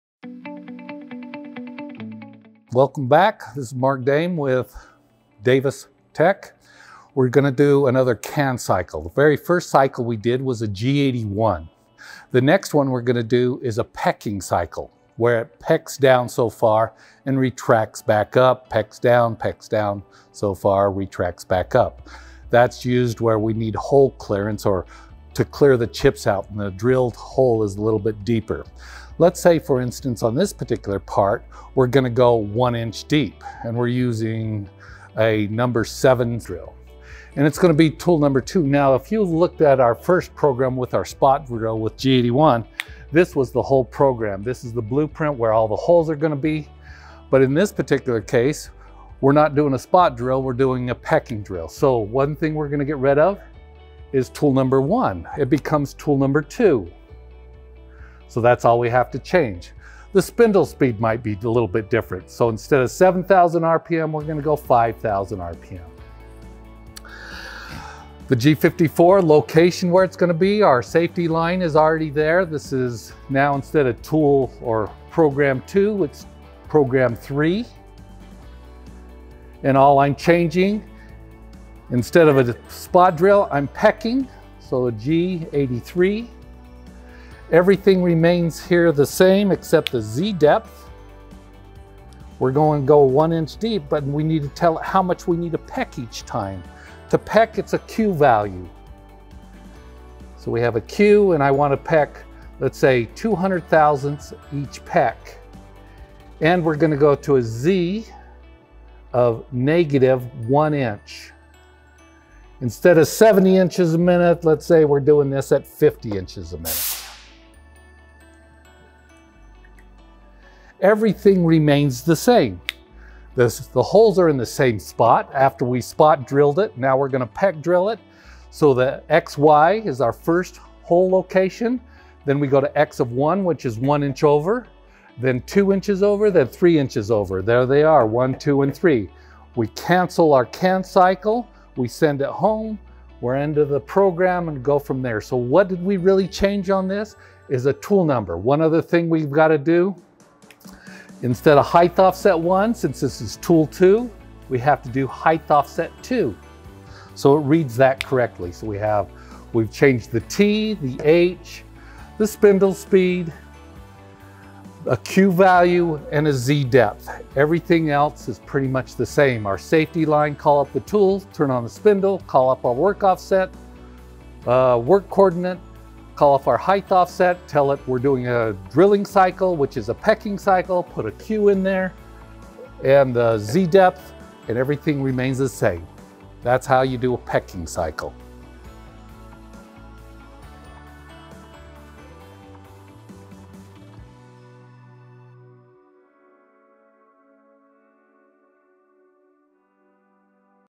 Canned cycles for the CNC Mill - G83 - Davis Technical College
Here is a video going over the G83 canned cycle for the CNC Mill